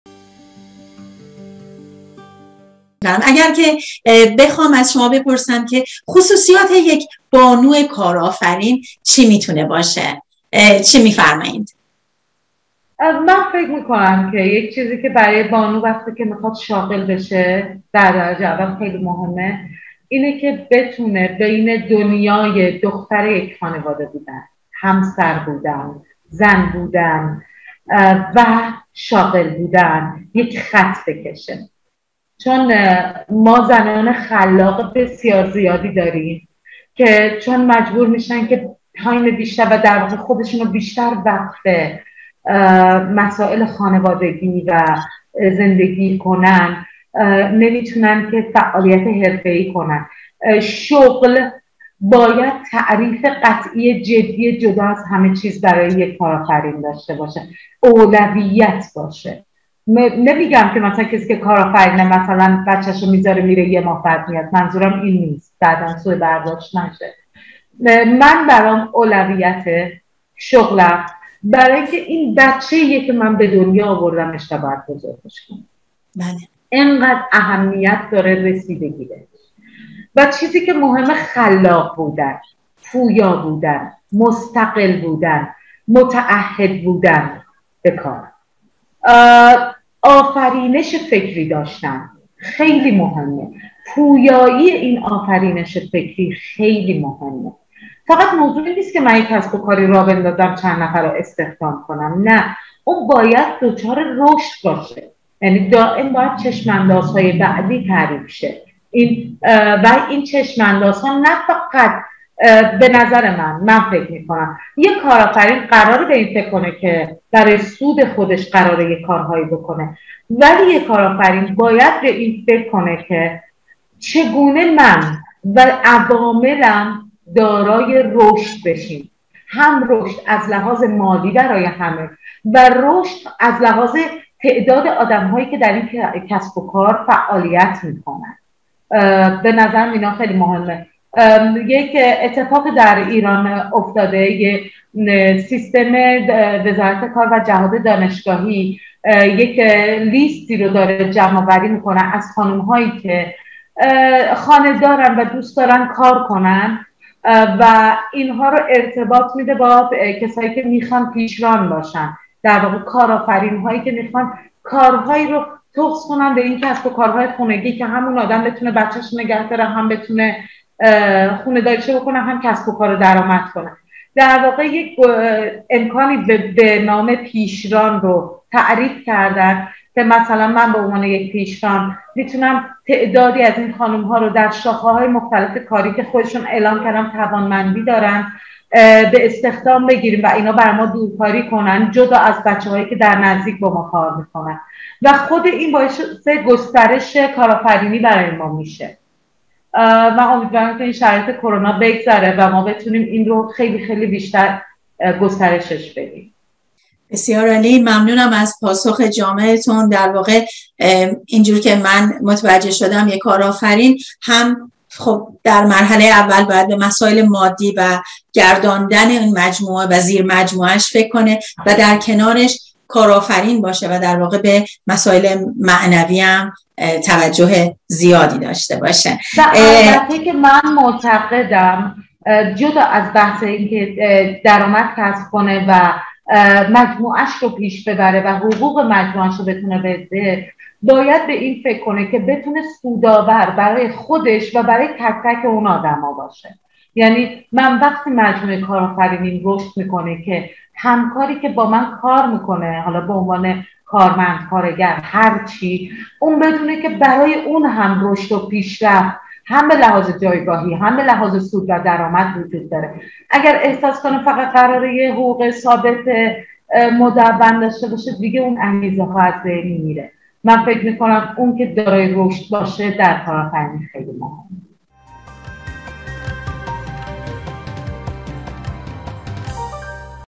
مروری بر مصاحبه با اکرم بایه - قسمت پنجم
Akram Bayeh Interview - Highlight - Part 5

میهمان: اکرم بایه
مجری: رز صانعی
موضوع: طراح لباس
Guest: Akram Bayeh
Host: Rose Sanei
Subject: Dress Designer

آهنگساز:
حامد سلطانی
Composer:
Hamed Soltani